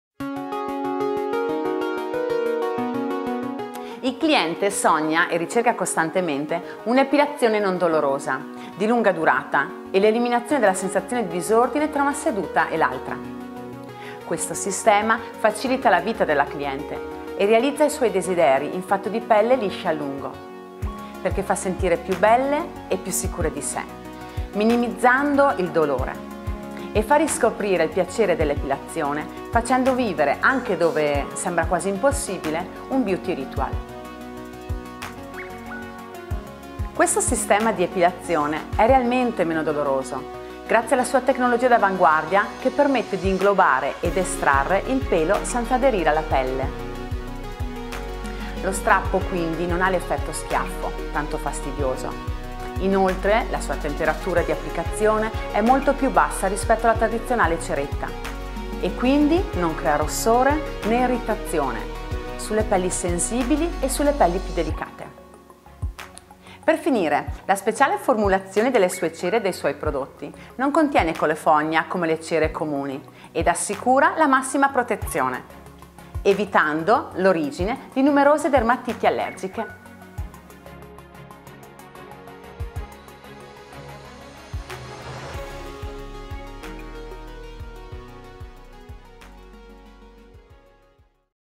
SKIN'S BEST BRAZILIAN WAXING - MYTHO Parrucchieri Estetica
MYTHO Parrucchieri Estetica racconta Skin's Best Brazilian Waxing!

"Ne sono un esempio le madri che, scoprendone la delicatezza, lo hanno voluto regalare alle figlie giovanissime come prima esperienza al servizio stesso; le donne che dopo un vissuto negativo di “fai da te”, hanno riscoperto il piacere di affidarsi di nuovo a mani professionali.." cit. Norma, titolare